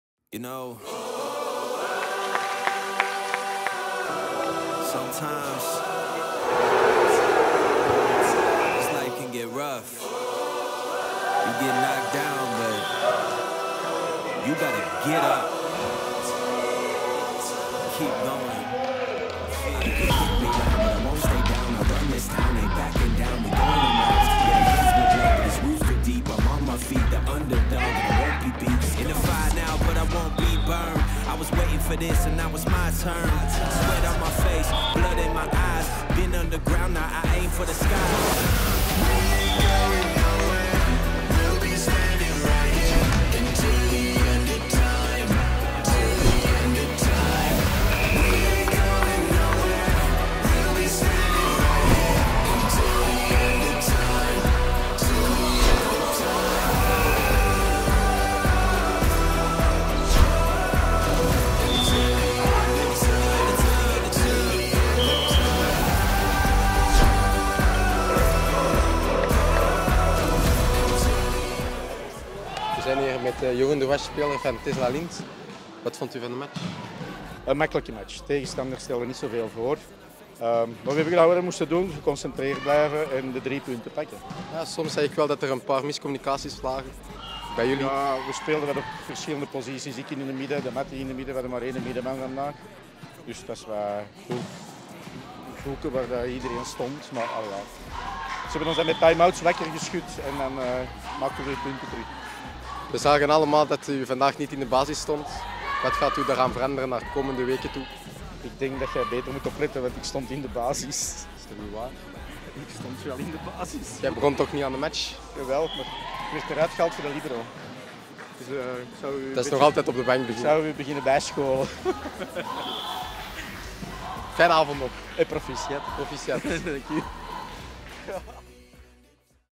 JDWSCH - Sveka Schoten - Tesla Lint
Tesla Lint went last friday to sports hall De Zeurt to face Sveka Schoten in a league match. They needed no effort to secure the win today.